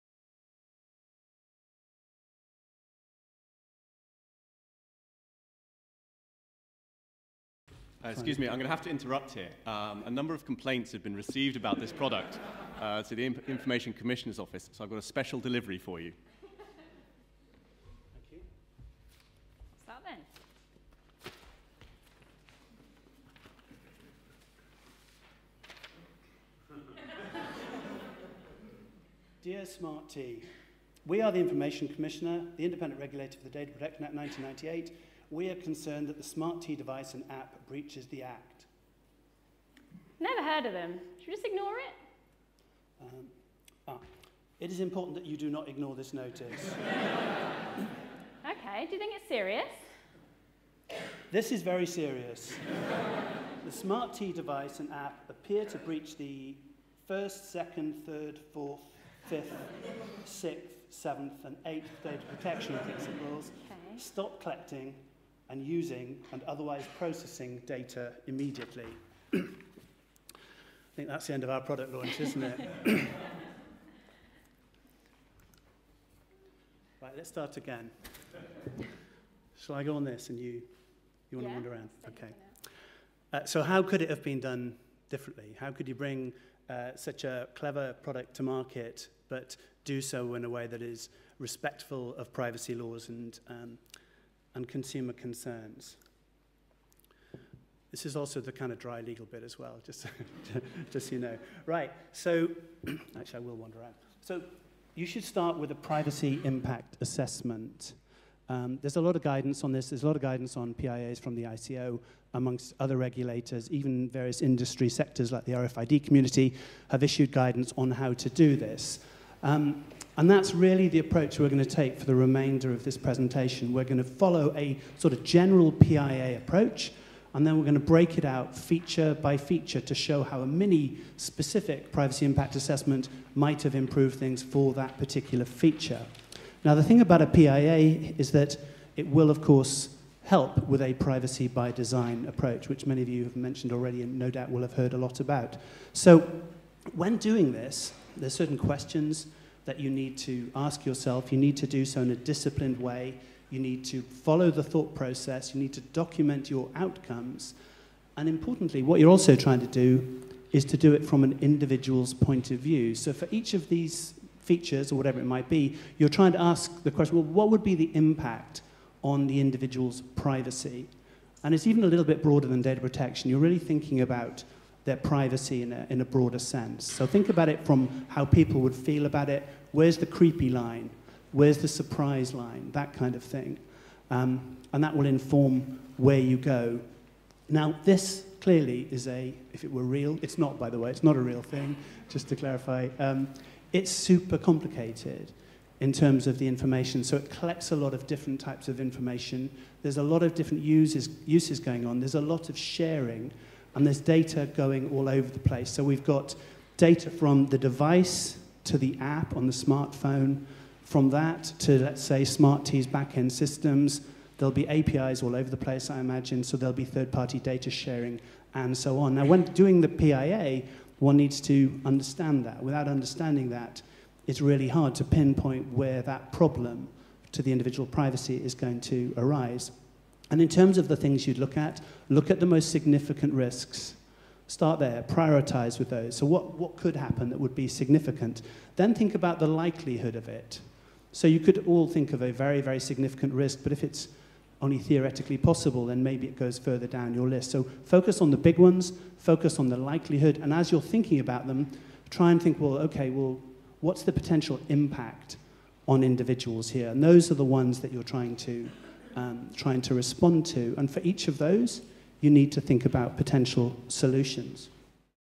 Wearable technologies: Privacy, data security, associated risks and recommendations
Speaker: Dr Mark Watts, Managing Partner, and Faye Harrison, Associate, Bristows, London
Recorded at the Privacy Laws & Business 28th Annual International Conference
Privacy in a Connected World
6 - 8 July 2015, Cambridge, UK

A case study looking at key privacy considerations when bringing new wearable technology to market:

• Current wearable trends – overview of features, functionality and capabilities

• Potential use cases – looking at data aggregation, interconnectivity and data sharing

• Key privacy risks in consideration of some of the more intrusive use cases for wearables

• Privacy by design – managing compliance risk when bringing new wearables to market

• General practical recommendations for compliance